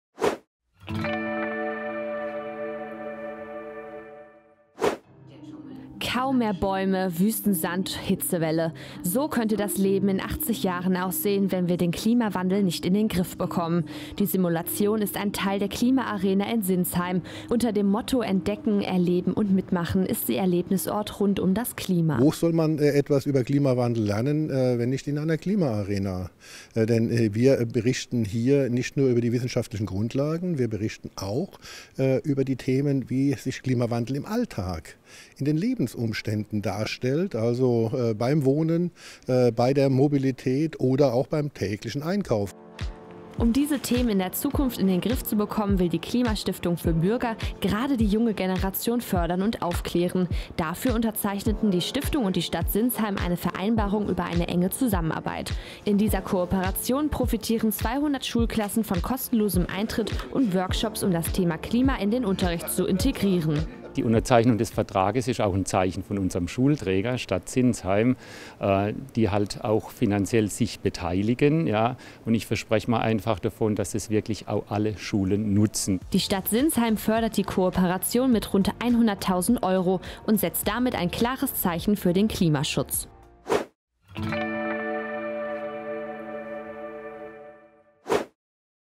Klima Arena in Sinsheim | RON TV
200 Schulklassen profitieren: Ein Thema das uns alle betrifft ist der Klimaschutz. Die Stadt Sinsheim ist deshalb seit heute Partner der Klima Stiftung für Bürger. Alleine 200 Schulklassen sollen von dem Projekt profitieren.